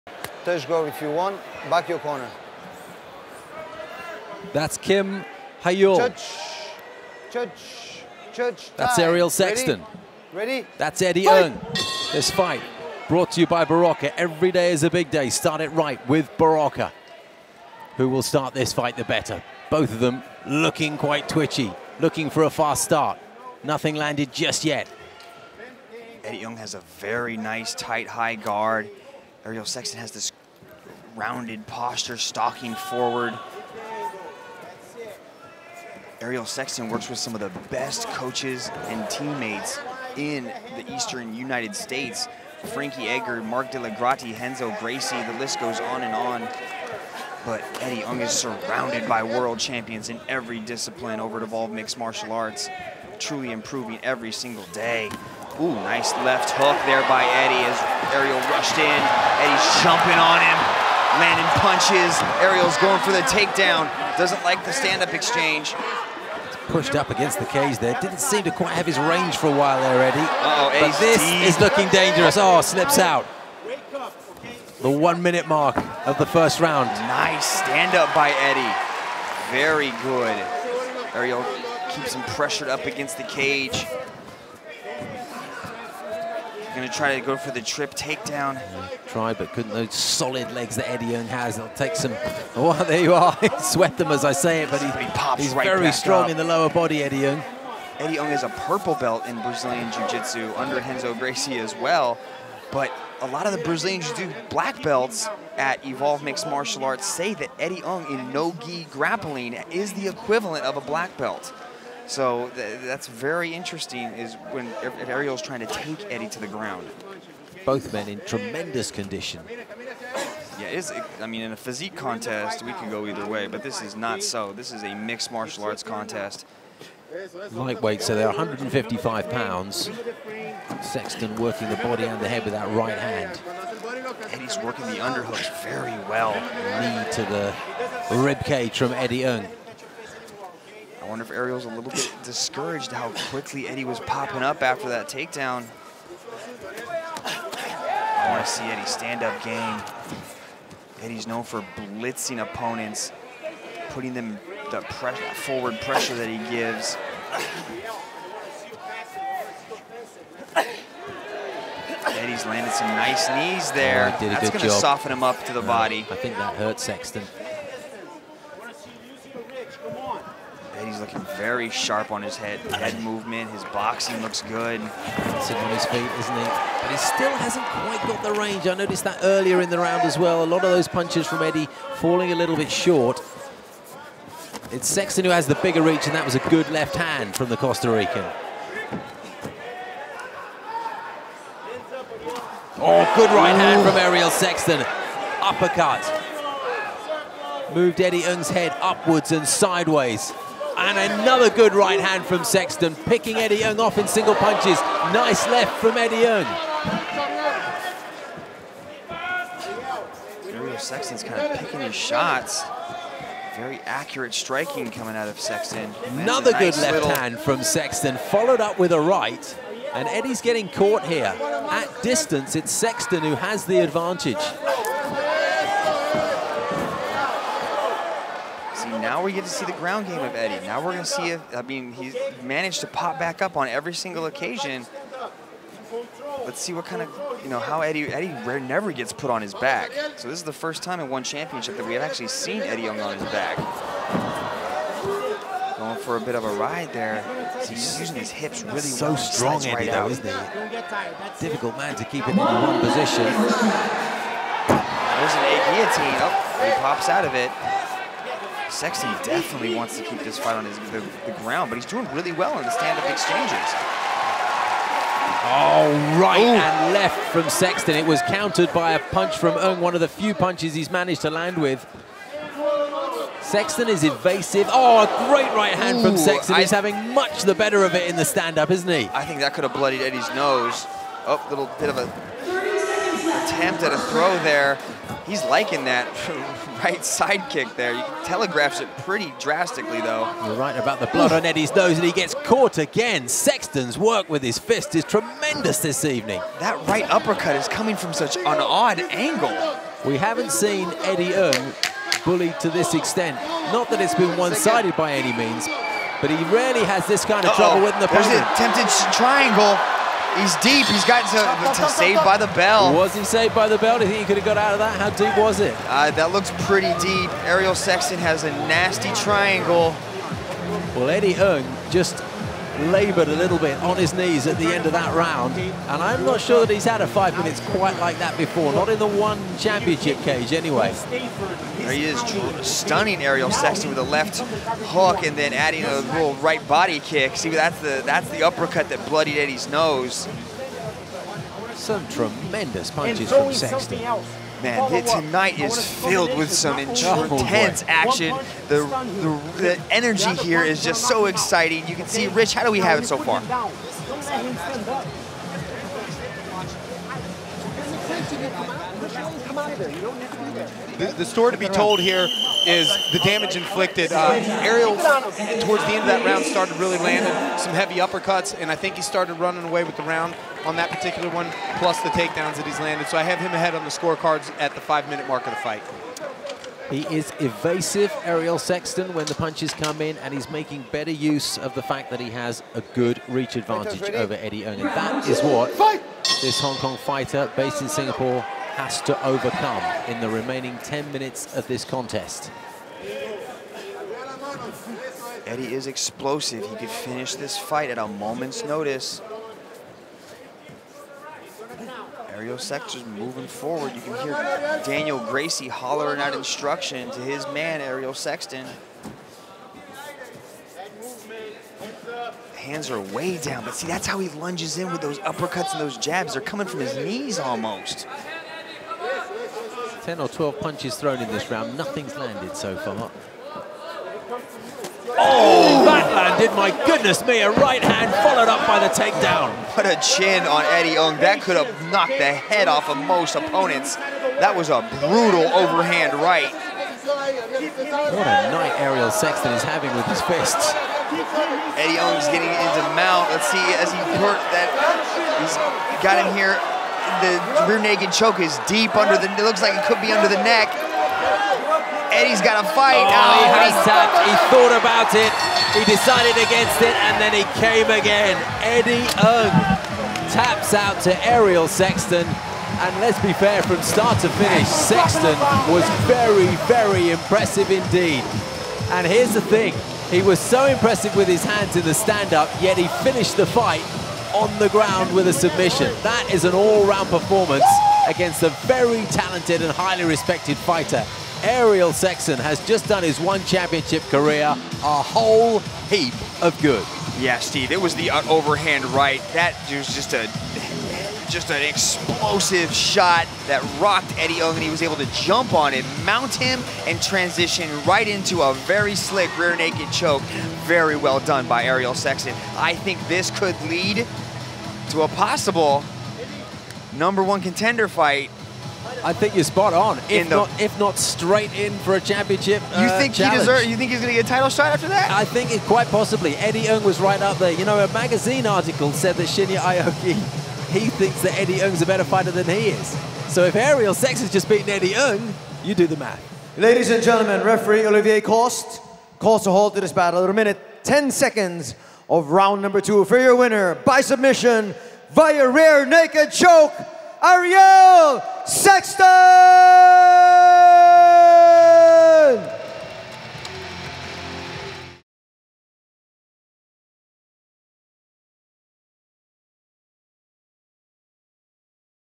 ONE: Full Fight | Ariel Sexton vs. Eddie Ng | An Opportunity Seized | May 2015
When two of the most exciting lightweights in ONE Championship clashed, it was going to be fireworks. Ariel "Tarzan" Sexton met Eddie "The Magician" Ng at ONE: WARRIOR’S QUEST from the Singapore Indoor Stadium. “Tarzan” seized the opportunity and announced himself to the rest of the lightweight division with a wild performance!